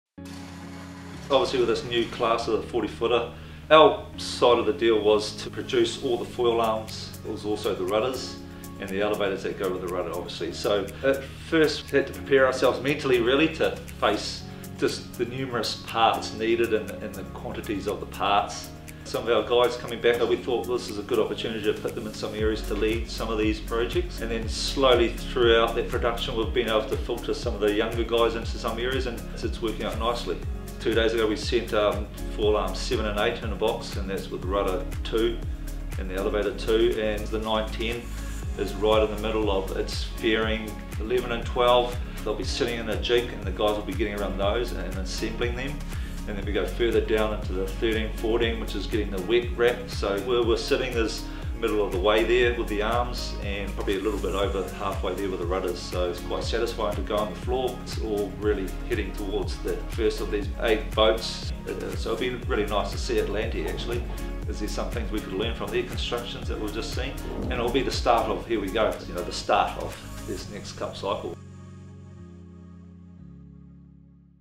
The Appendage Factory
The Emirates Team New Zealand build facility on Auckland’s North Shore has been running at capacity for the past 8 months building all of the foils and appendages that will allow the AC40 to fly.